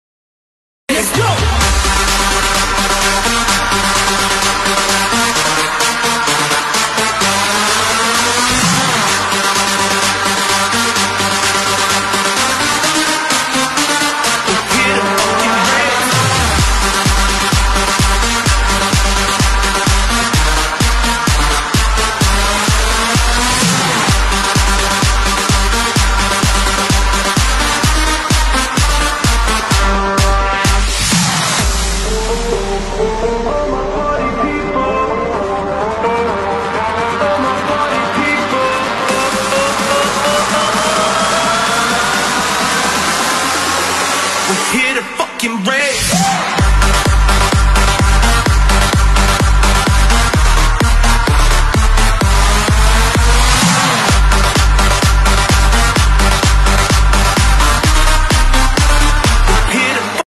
Day 1 Ohrid Calling 2019 after Movie 1st August 2019
DAY 1- ✔️ Yesterday was lit 🔥, today is going to be even better! Shoutout to @steveaoki! @mauricewest, @hugelthug , @djfelicity and our @galoskimusic for a hell of a show!  Buy your tickets on avalon.mk, the Ohrid city square or the box office at the venue!